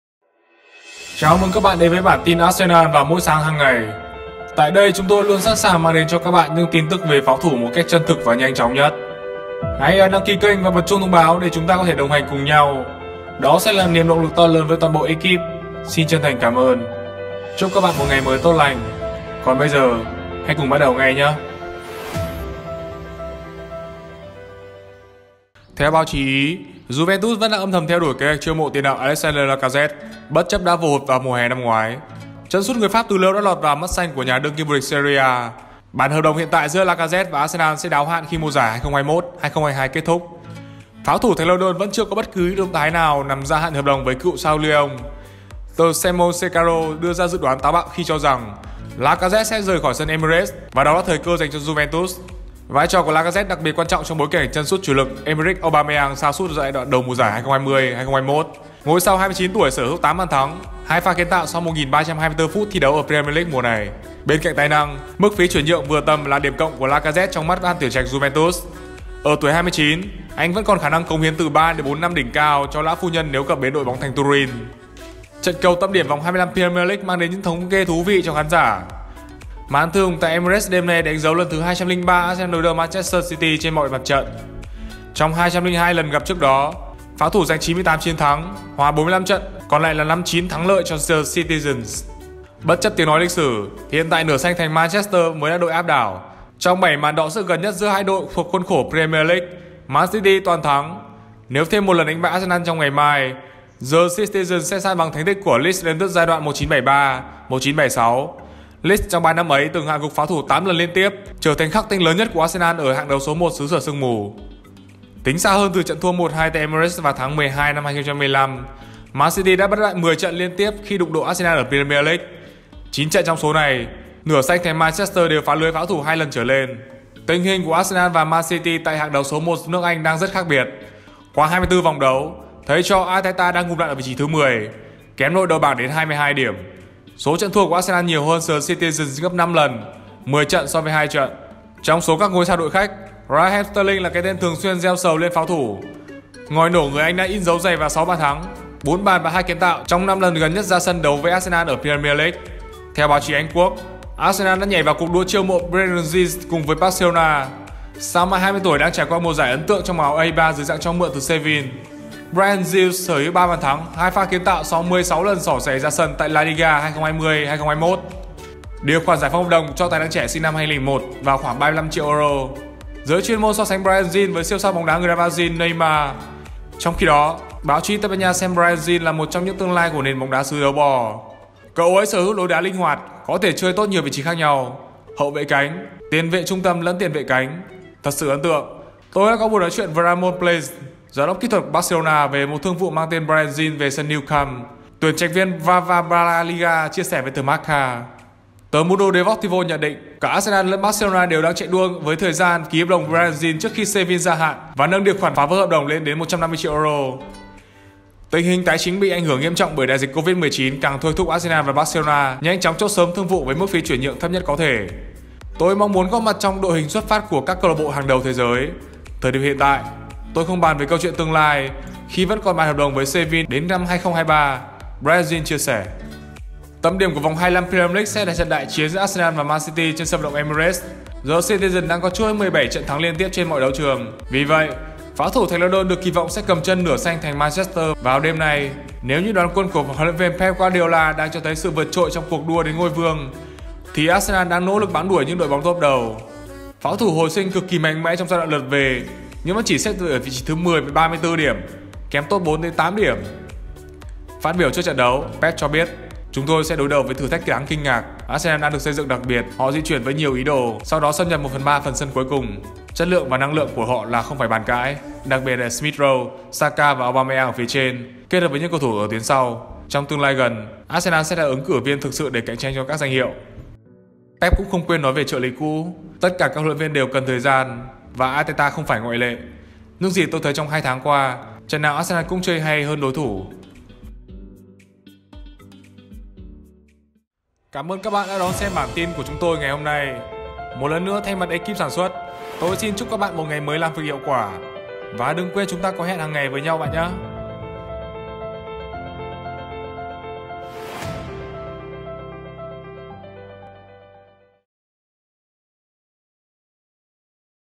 BẢN TIN ARSENAL 21/2 | NHỮNG CON SỐ HÂM NÓNG ĐẠI CHIẾN | JUVENTUS TÍNH CƯỚP SAO ARSENAL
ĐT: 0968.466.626

© Bản quyền thuộc về The Gunners